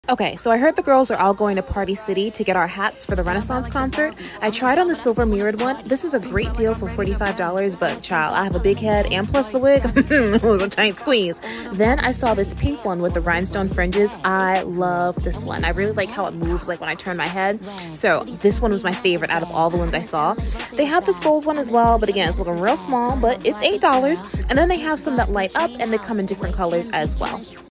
Beyonce Renaissance Concert Cowboy Hats 🐝🪩✨
Are you heading to the Beyoncé Renaissance concert and looking for the perfect cowboy hat to complete your outfit? 🐝🪩✨ Look no further! In this video, I’ll show you where to find the best cowboy hats for the concert, can you believe Party City is the plug?!

Whether you're a die-hard Beyoncé fan or just love western fashion, this video is for you. Don't miss out on the chance to rock the perfect cowboy hat at the Beyoncé Renaissance concert!